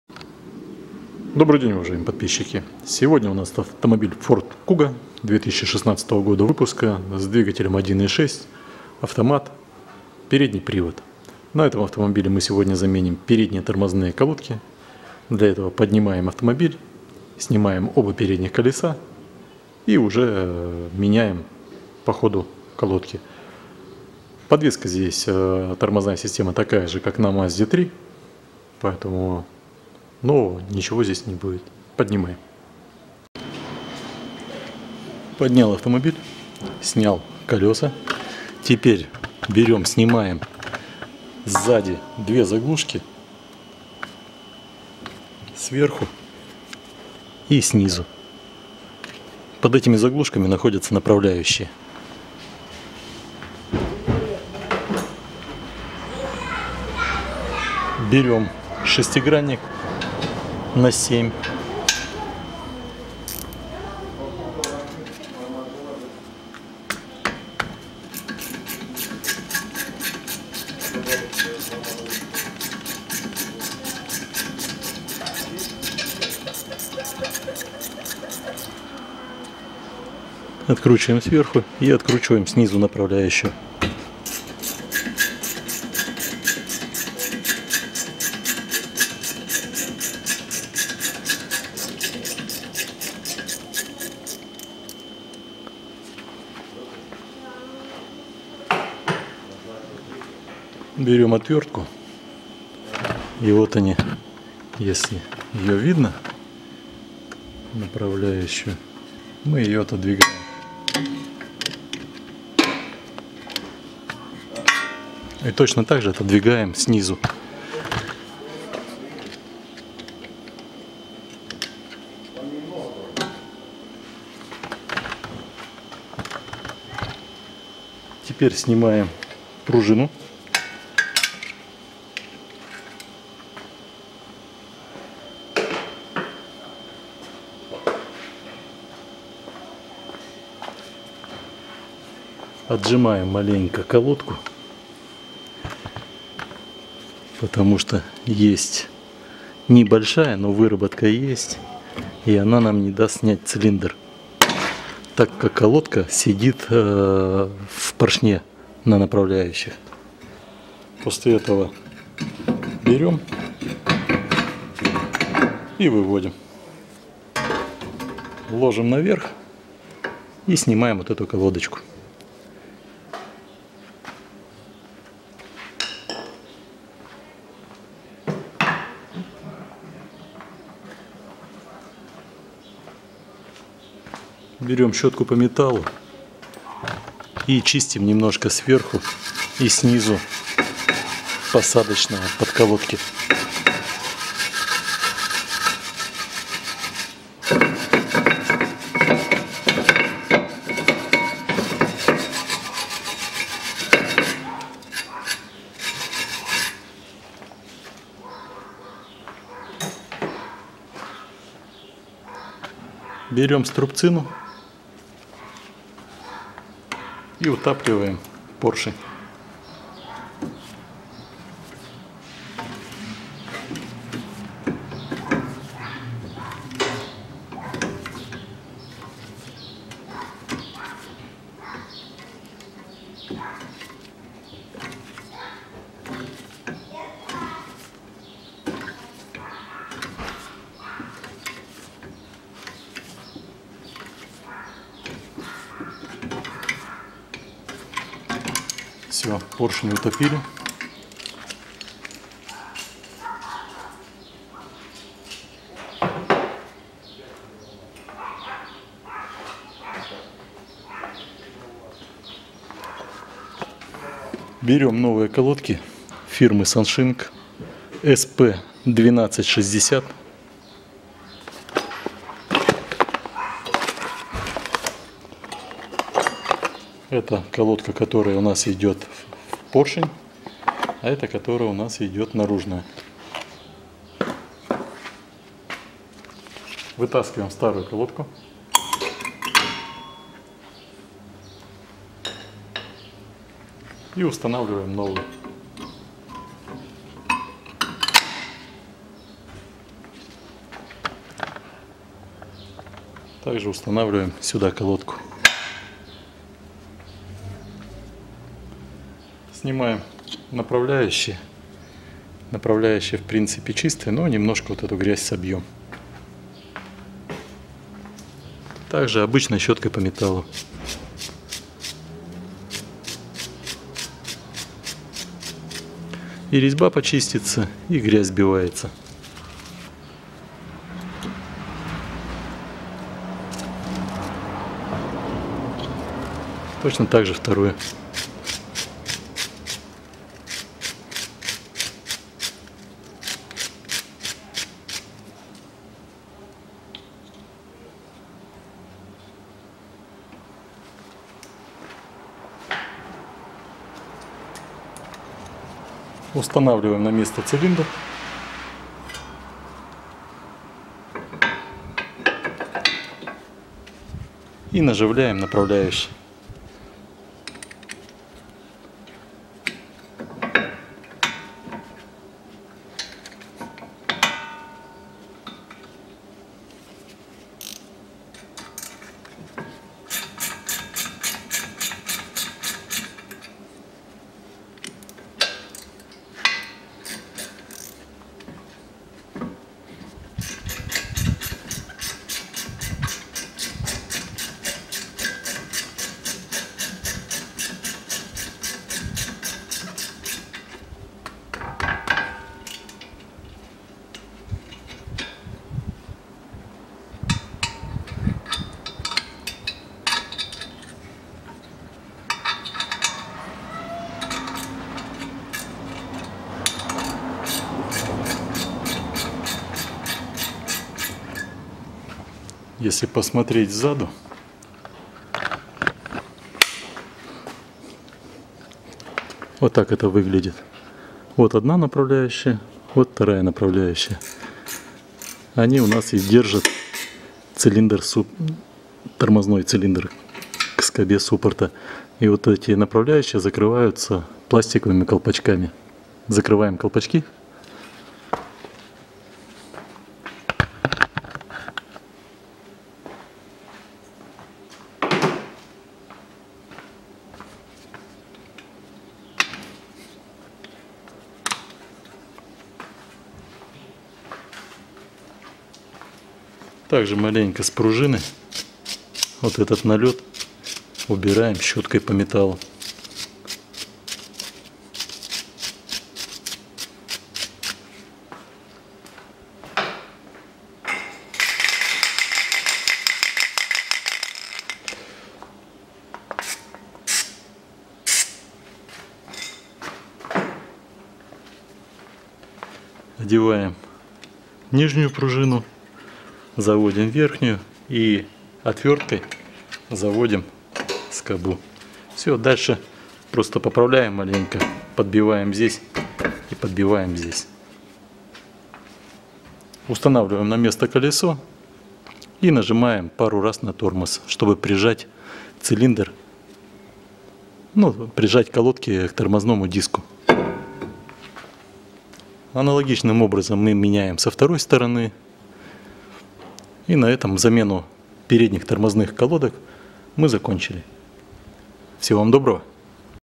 Ford Kuga 1,6 Форд Куга 2016 года Замена передних тормозных колодок
тормозные колодки  SANGSIN  SP-1260